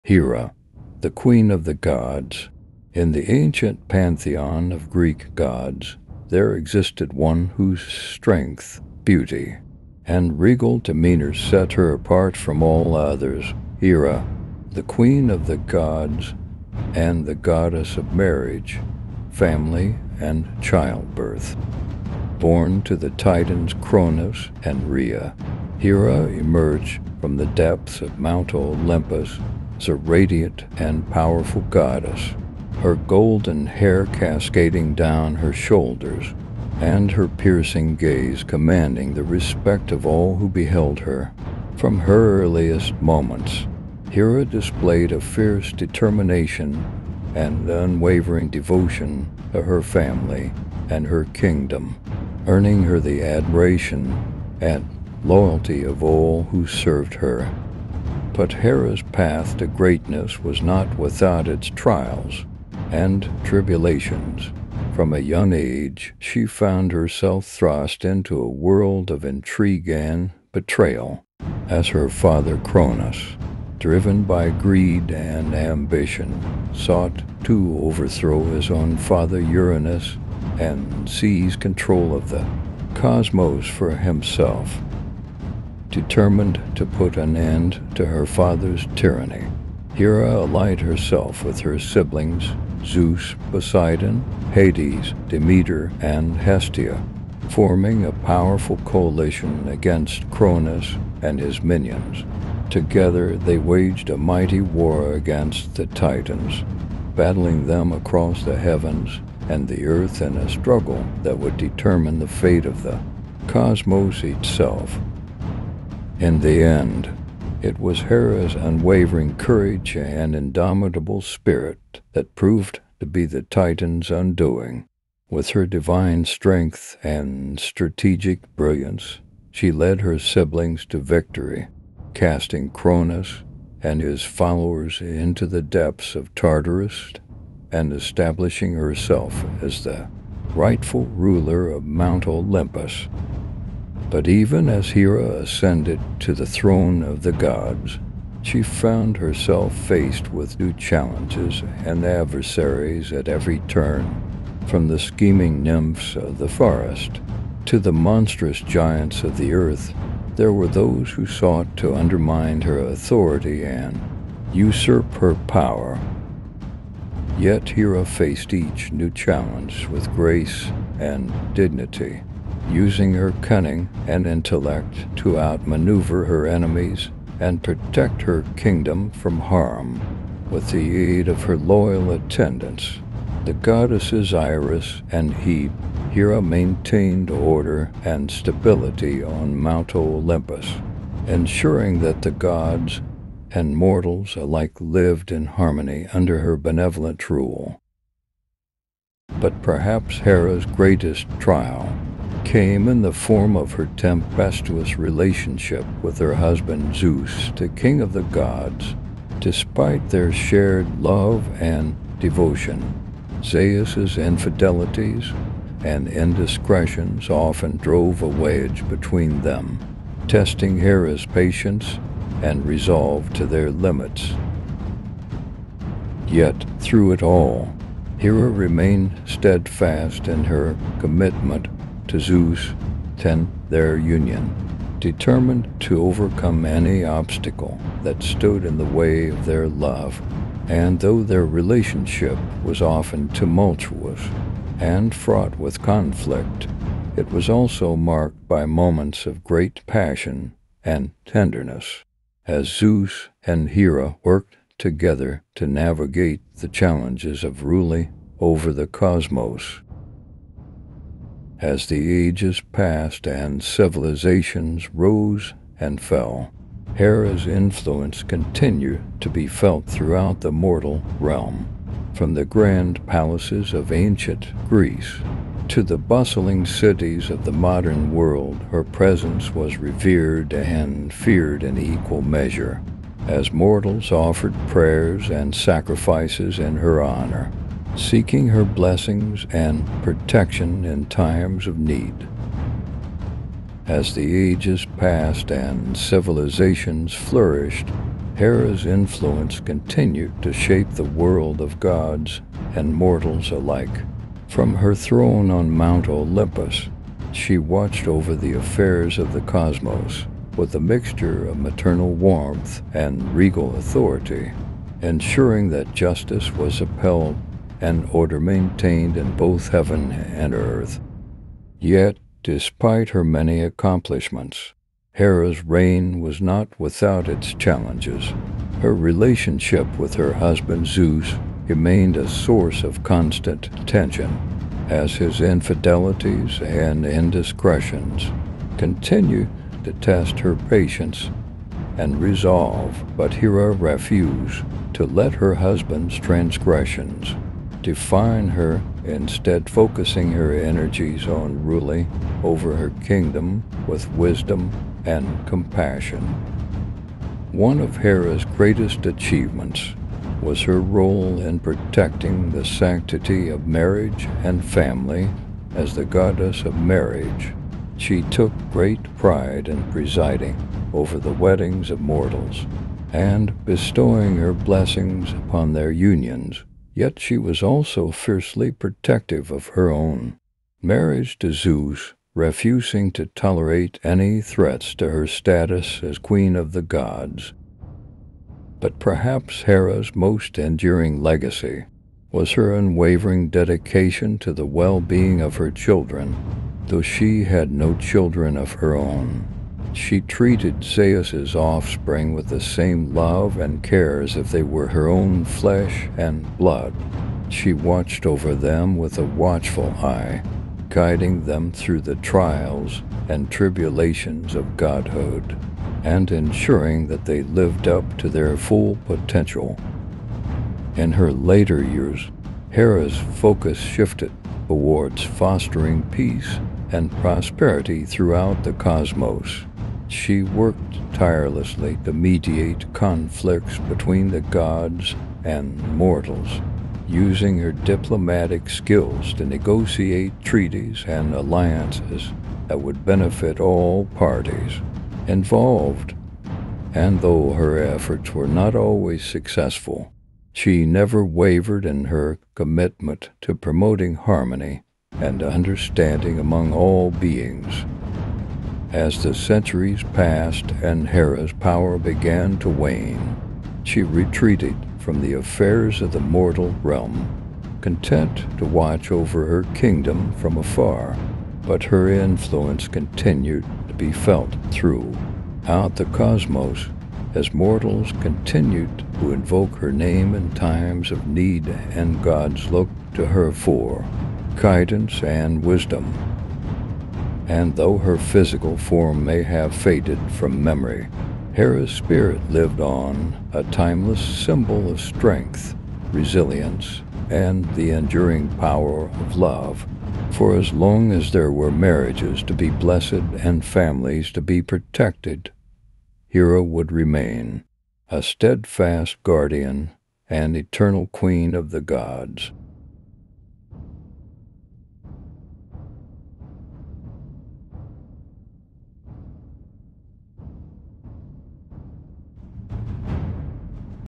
Hera
Hera: The Queen of the Gods

In the ancient pantheon of Greek gods, there existed one whose strength, beauty, and regal demeanor set her apart from all others - Hera, the queen of the gods and the goddess of marriage, family, and childbirth.

Born to the titans Cronus and Rhea, Hera emerged from the depths of Mount Olympus as a radiant and powerful goddess, her golden hair cascading down her shoulders and her piercing gaze commanding the respect of all who beheld her. From her earliest moments, Hera displayed a fierce determination and unwavering devotion to her family and her kingdom, earning her the admiration and loyalty of all who served her.

But Hera's path to greatness was not without its trials and tribulations. From a young age, she found herself thrust into a world of intrigue and betrayal, as her father Cronus, driven by greed and ambition, sought to overthrow his own father Uranus and seize control of the cosmos for himself.tsapp